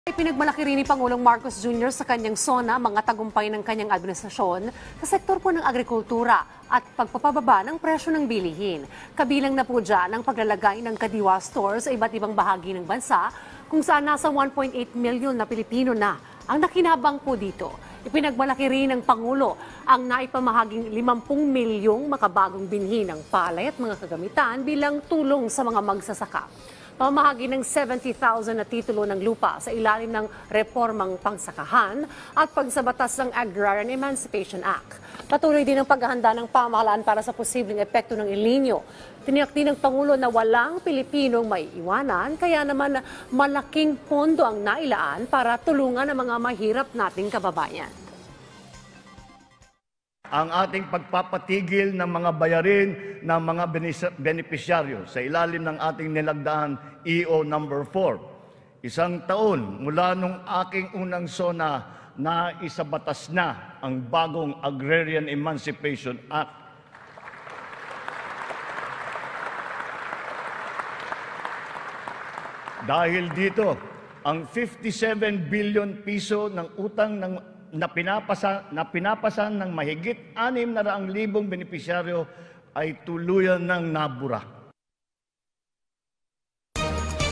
PBBM, ipinagmalaki ang mga tagumpay sa sektor ng agrikultura at pagpapababa ng presyo ng bilihin
PBBM, ipinagmalaki rin ang naipamahaging tulong sa mga magsasaka;

Pagsasabatas ng Agrarian Emancipation Act, nabanggit din sa SONA;

Patuloy na paghahanda sa posibleng epekto ng El Niño, natalakay rin sa SONA